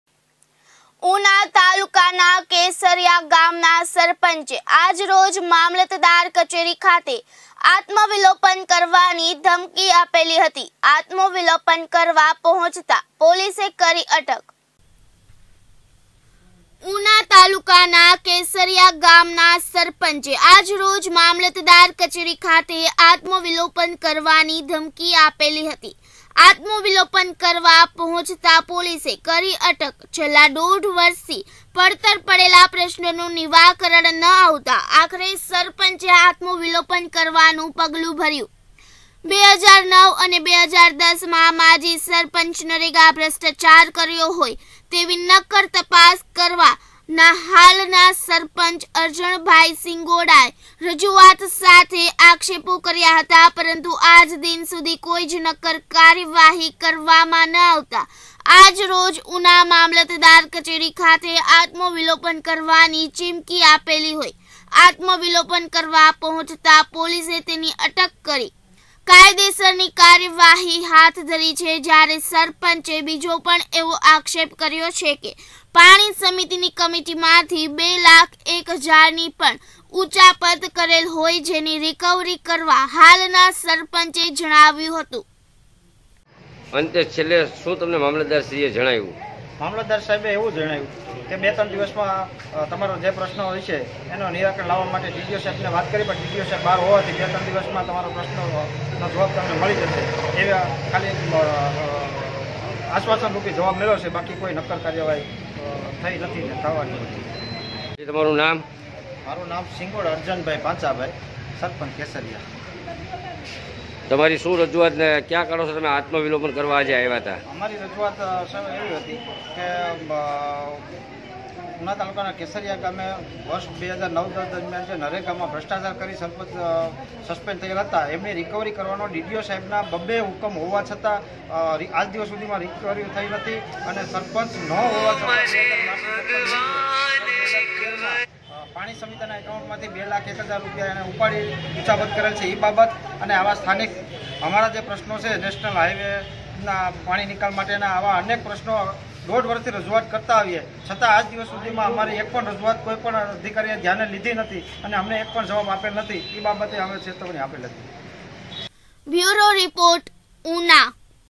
ઉના તાલુકાના કેસરિયા ગામ ના સરપંચે આજરોજ મામલતદાર કચેરી ખાતે આત્મવિલોપન કરવાની ...
ઉના તાલુકાના કેસરિયા
ગામ ના સરપંચે આજરોજ
મામલતદાર કચેરી ખાતે
આત્મવિલોપન કરવાની
ધમકી આપેલી હતી
આત્મવિલોપન કરવા
પહોંચતાં પોલીસે કરી અટક